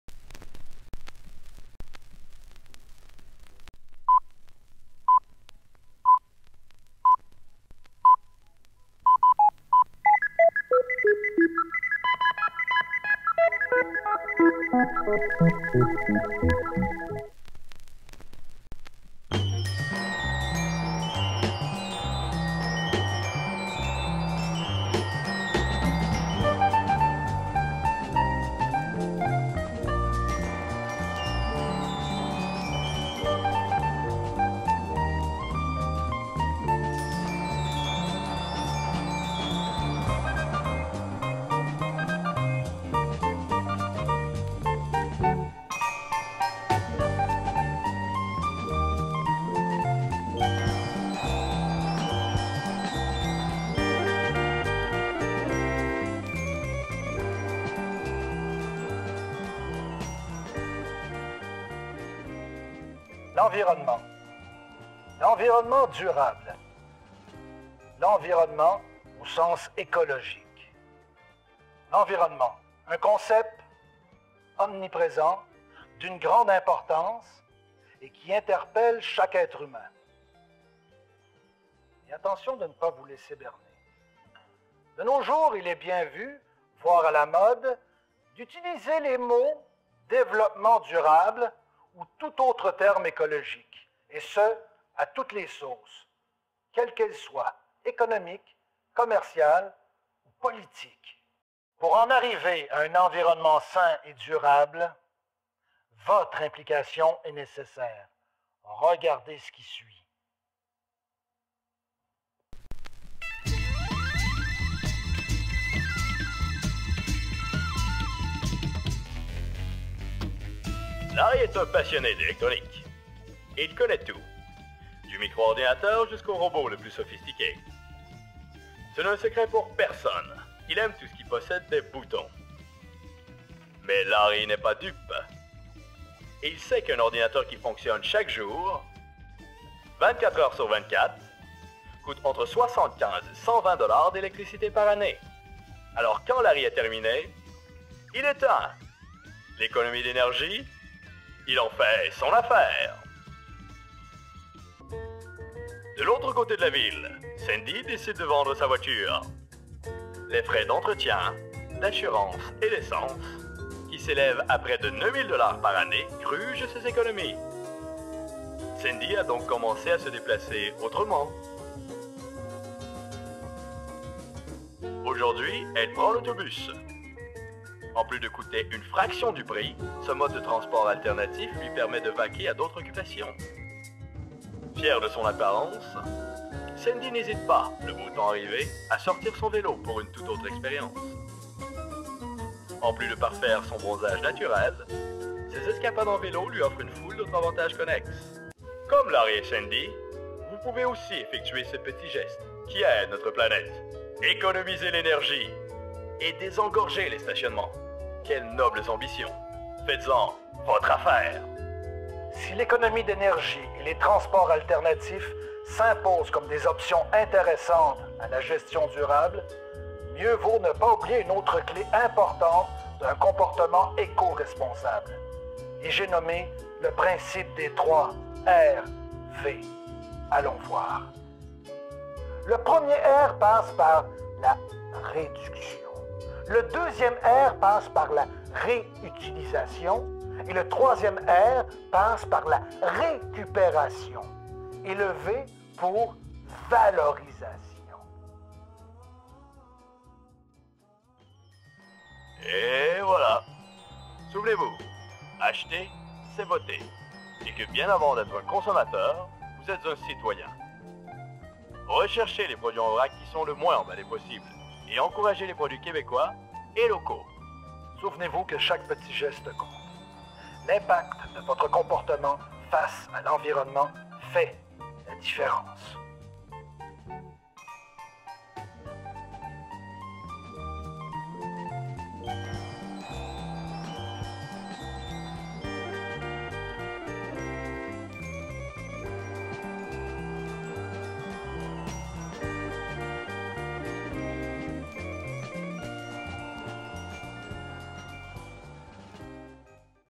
En Vert et pour tous
Créé avec un style rétro, ce court métrage présente deux protagonistes dans leur milieu dont  un « scientifique » qui explique aux citoyens des méthodes simples et efficaces de préserver l'environnement. C'est un appel à une meilleure conscientisation collective et environnementale.